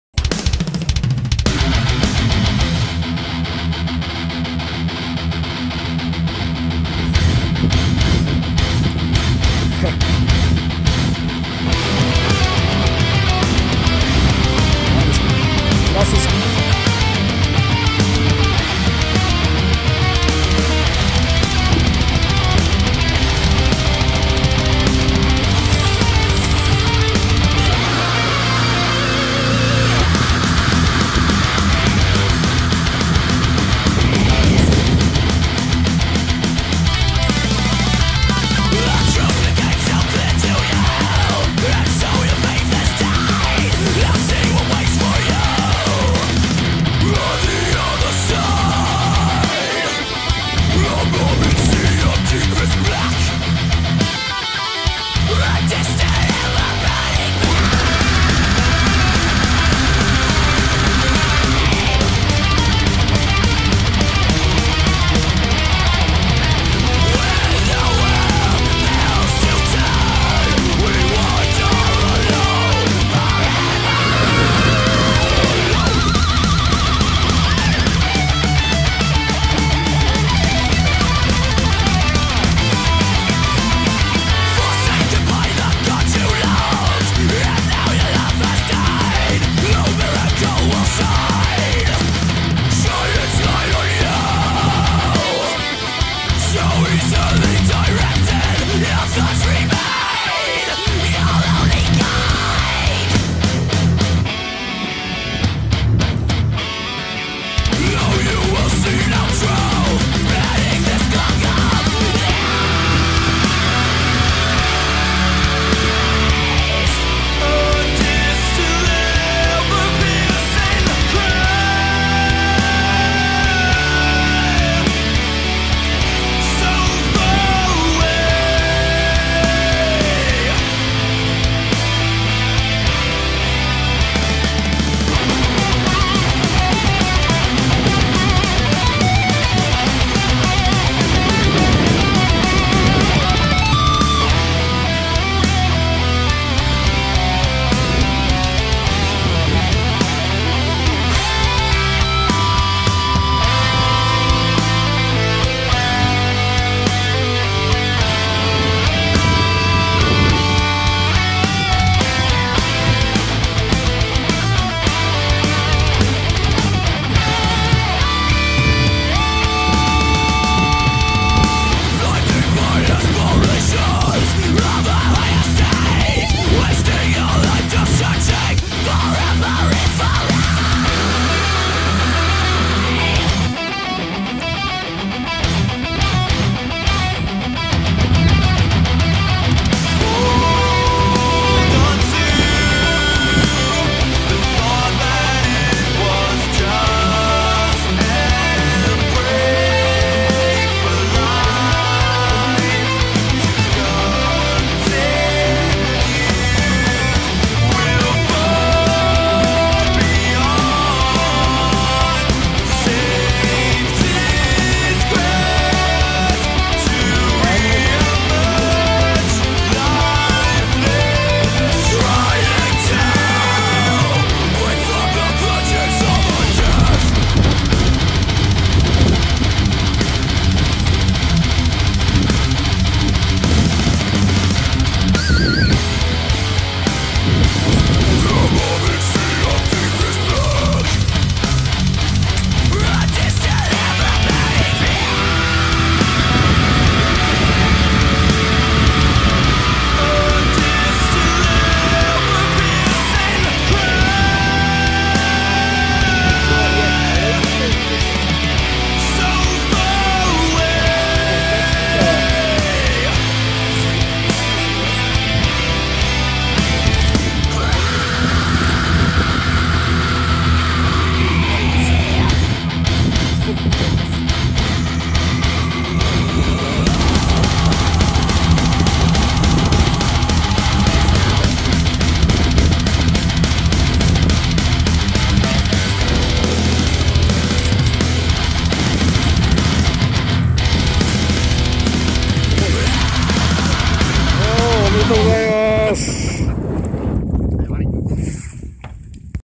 タイラバで83cmヒラマサ!!「Death Metal Fishing Boat GOD」
タイラバでヒラマサ!!83cm～渡邊さんおめでとうございます!!(^o^)／~~